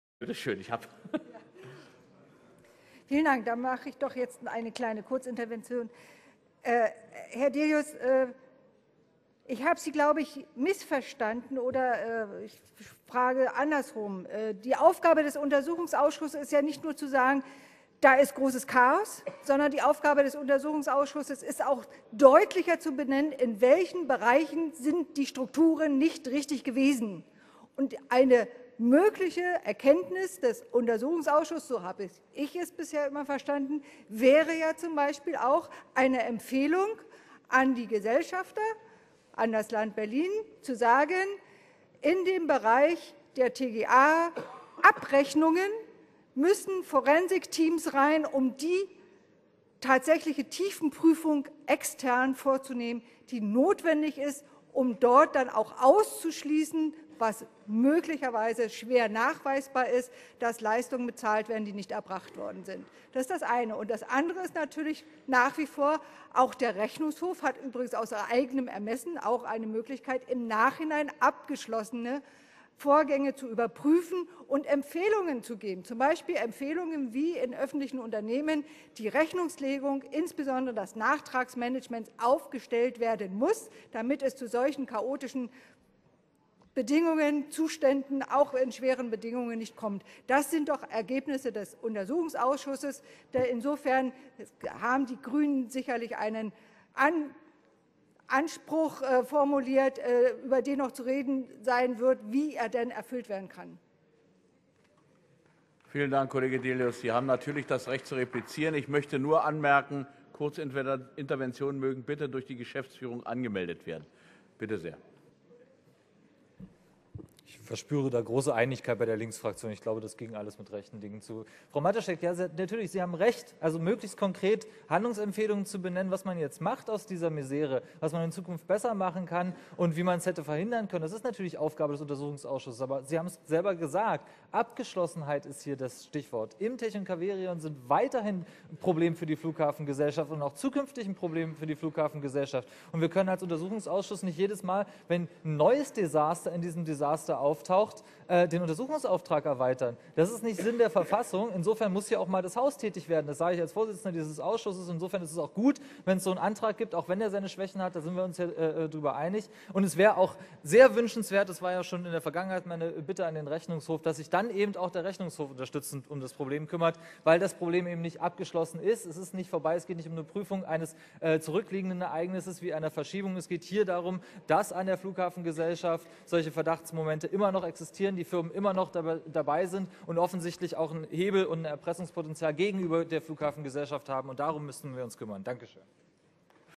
61. Sitzung - Matuschek vs. Delius zu Korruption am BER bekämpfen
12. März 2015
61. Sitzung des Berliner Abgeordnetenhauses
 Jutta Matuschek (Die Linke) vs. Martin Delius (Piraten)

Matuschek sieht die Aufgabe des BER-Untersuchungsausschusses auch darin zu benennen, in welchen Bereichen es fehlerhafte Strukturen gibt und Handlungsempfehlungen an das Land Berlin auszusprechen. Auch der Rechnungshof habe diese Möglichkeit bei bereits abgeschlossene Vorgängen. - Delius gibt Matuschek Recht. Handlungsempfehlungen seien natürlich Aufgabe des Untersuchungsausschusses. Aber es sei auch "Abgeschlossenheit" wichtig. Der Ausschuss könne nicht jedes Mal seinen Untersuchungsauftrag erweitern, sobald ein neues Desaster auftauche. Deshalb müsse "auch mal das Haus tätig werden" und sich der Rechnungshof unterstützend um das Problem kümmern.